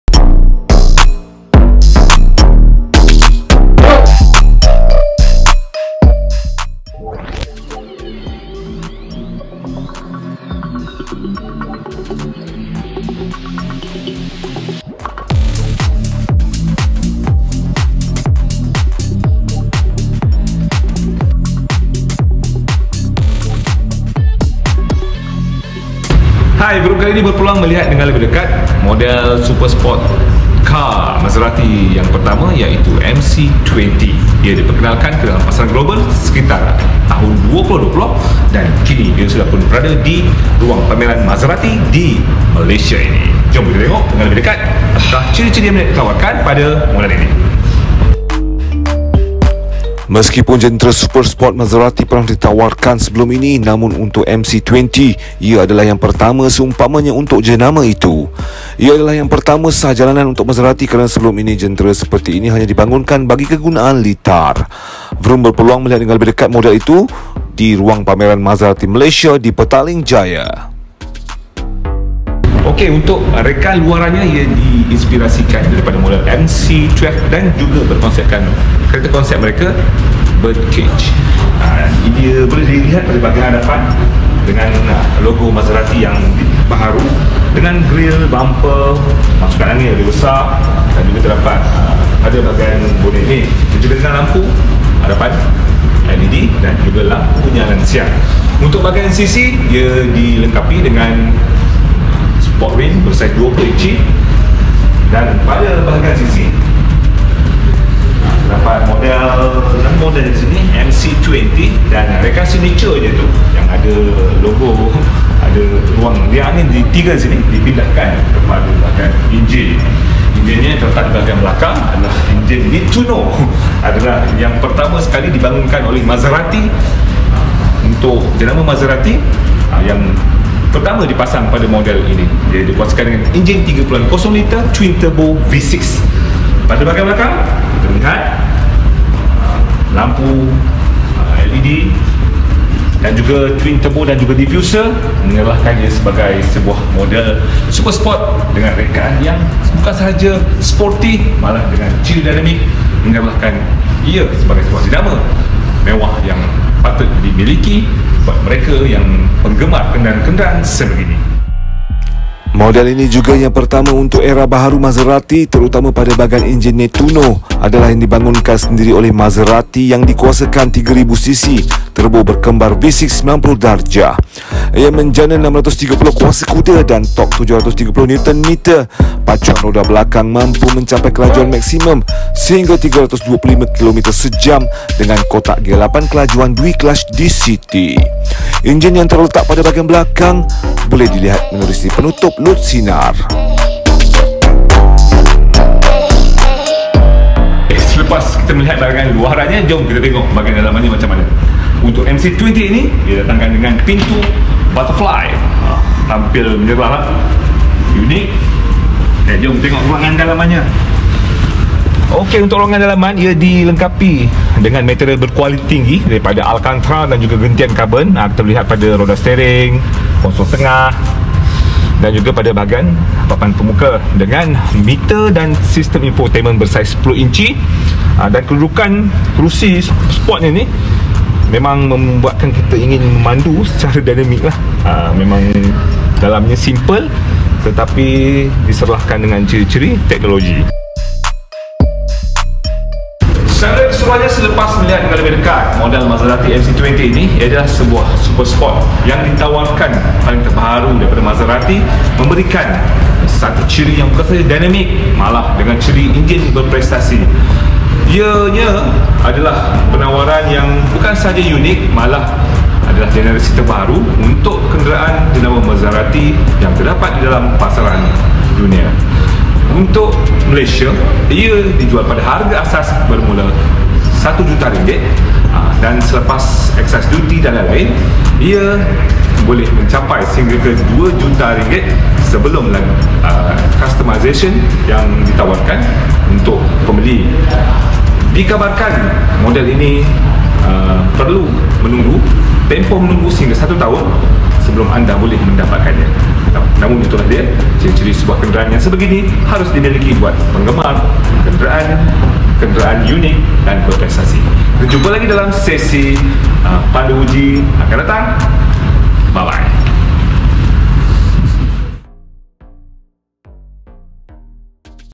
Maserati MC20 super sports sah jalananan bermula RM1.08 juta
Meskipun jentera super sport Maserati pernah ditawarkan sebelum ini namun untuk MC20 ia adalah yang pertama seumpamanya untuk jenama itu.

  Ia adalah yang pertama sah jalanan untuk Maserati kerana sebelum ini hanya dibangunkan bagi kegunaan litar.

  Vroom berpeluang melihat dengan lebih dekat model itu baru-baru ini di Ruang Pameran Maserati Malaysia di Petaling Jaya.